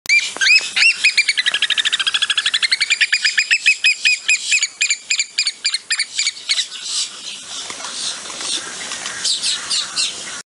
Mr. Peepers the American Oystercatcher
This bird was  loved by everyone who met him. He died in 2014.